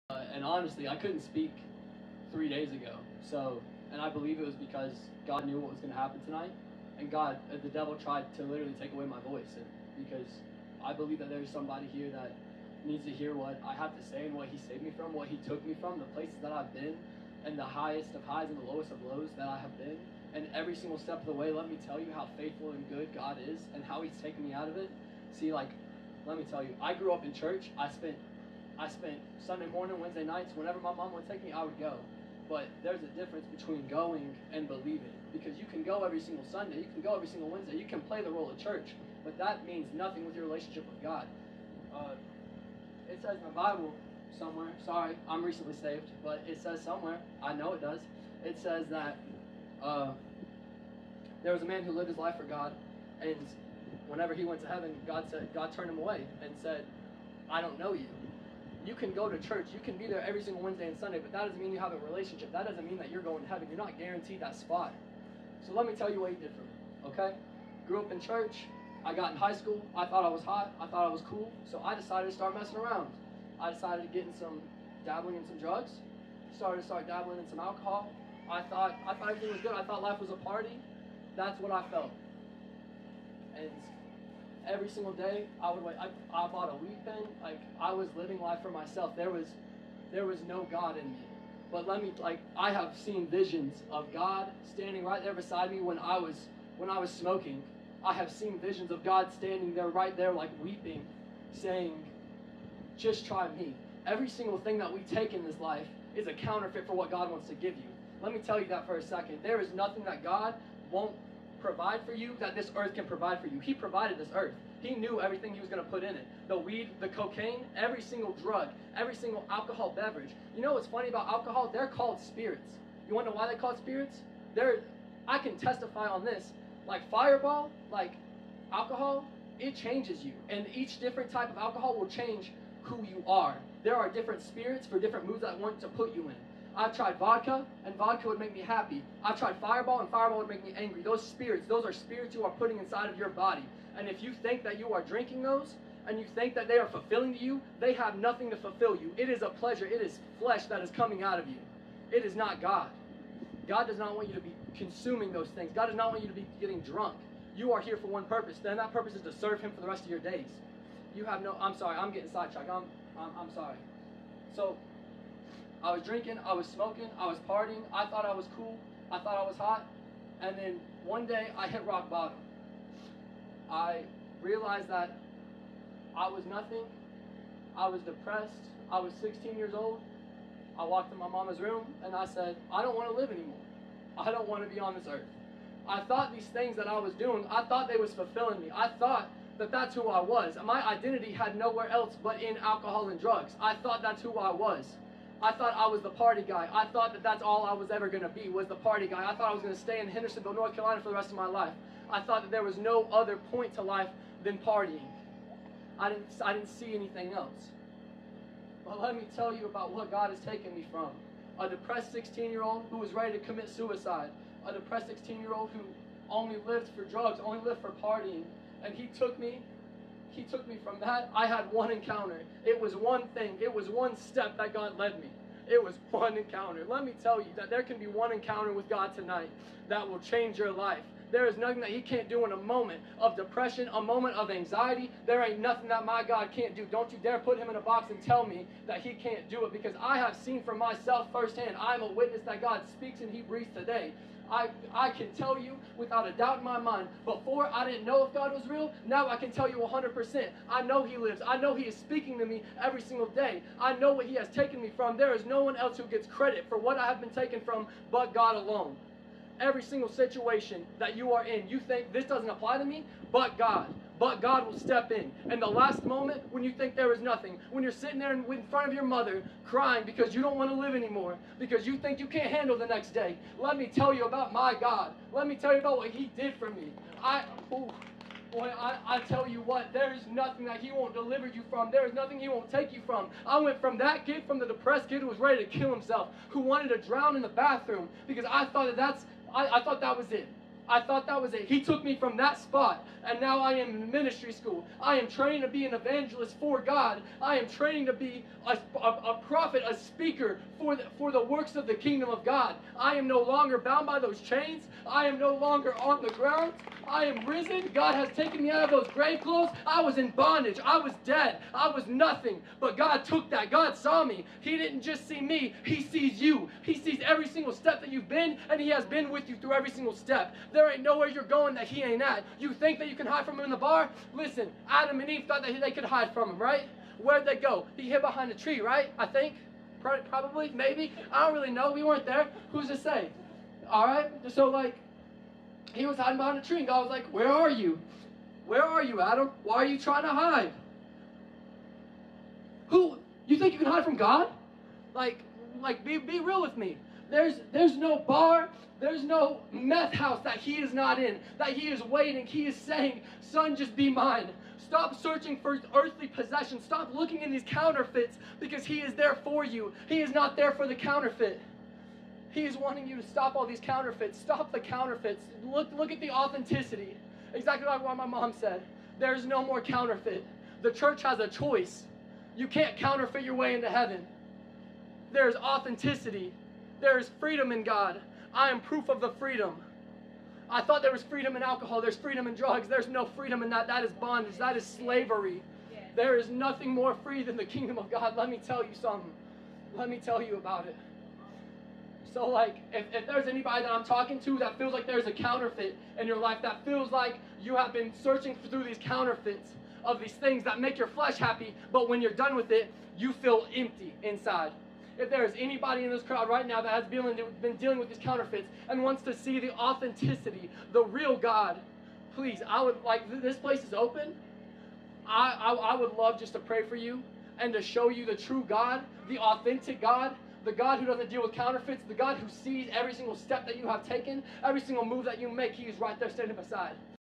Seth's Testimony
Our youngest son Seth giving his testimony for the first time, 11/13/21